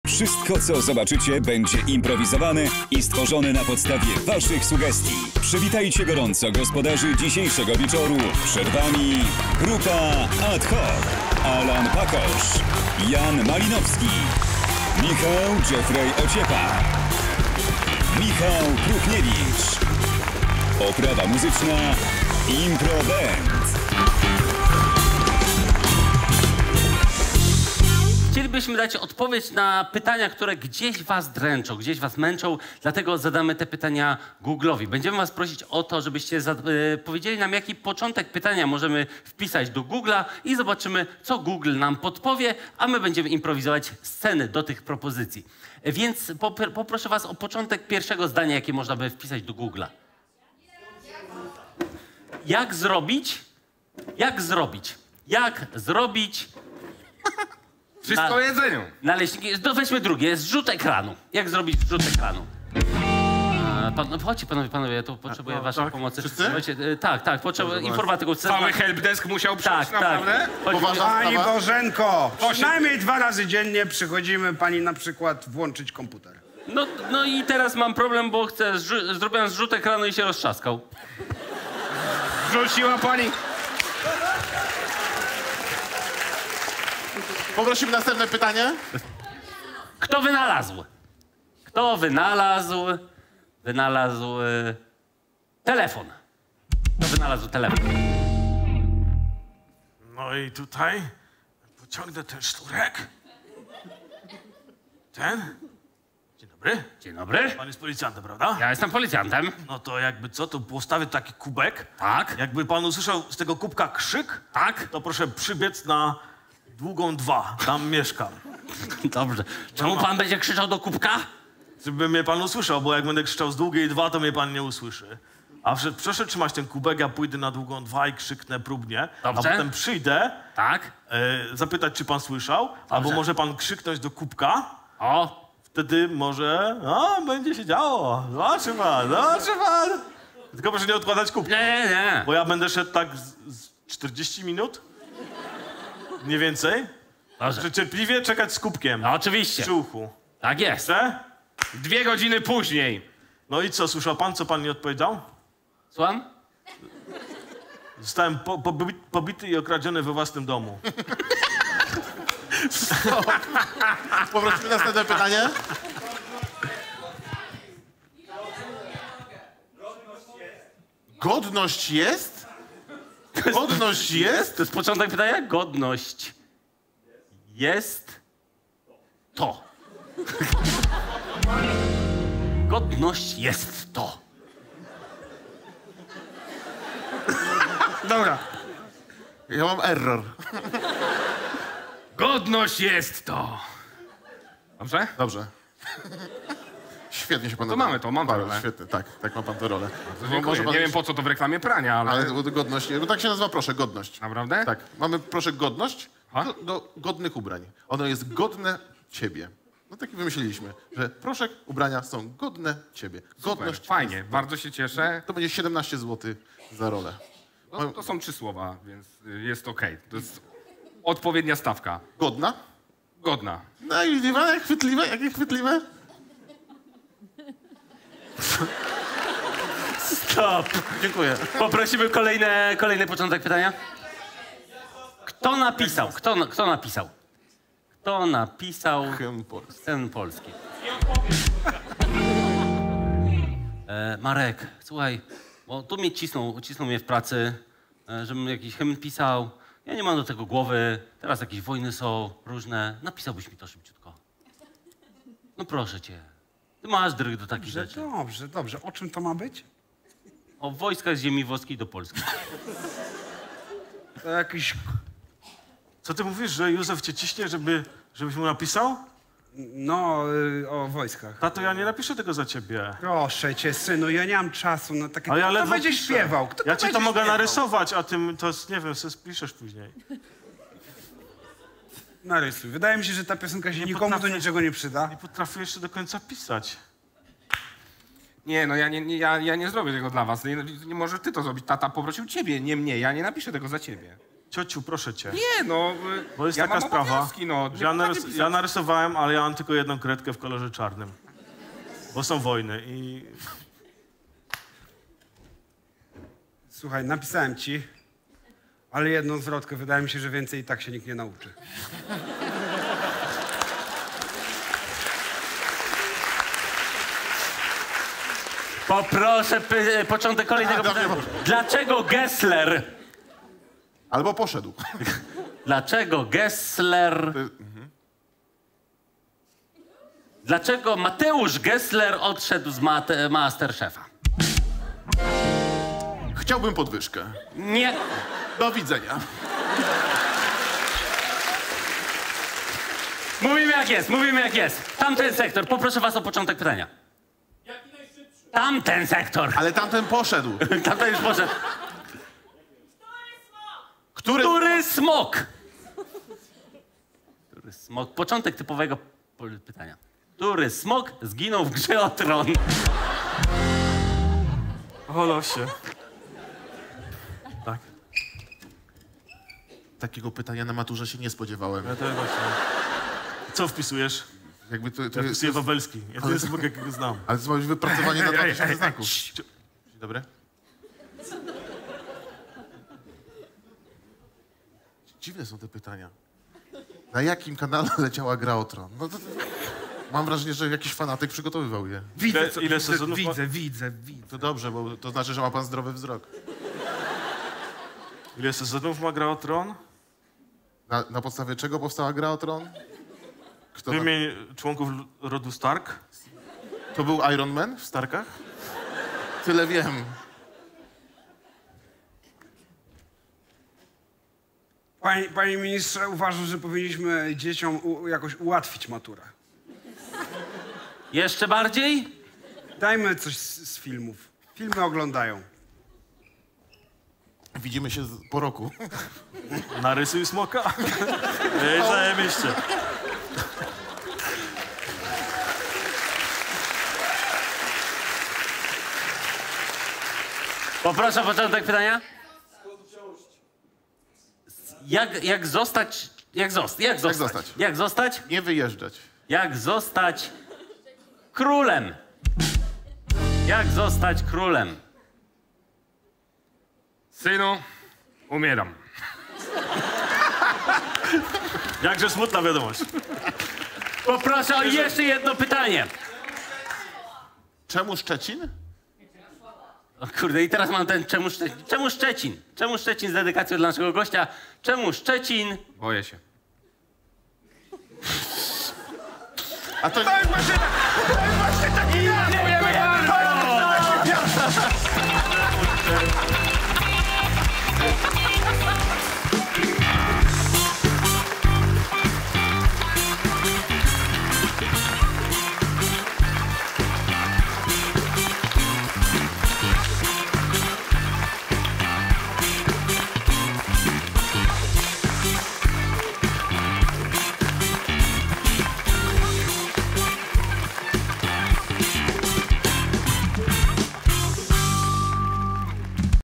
Wyszukiwarka #1 - Grzegorz Dolniak
W PEŁNI IMPROWIZOWANY WIECZÓR KOMEDIOWY TWORZONY NA PODSTAWIE SUGESTII WIDOWNI

⭐️ GRUPA AD HOC
▸ Alan Pakosz
▸ Michał Próchniewicz
▸ Janek Malinowski
▸ Michał Ociepa

🎥 REALIZACJA I MONTAŻ:
👉 MorningStar Media

Materiał zrealizowany podczas VARIETE’s Impro Show w krakowskim Teatrze Variete.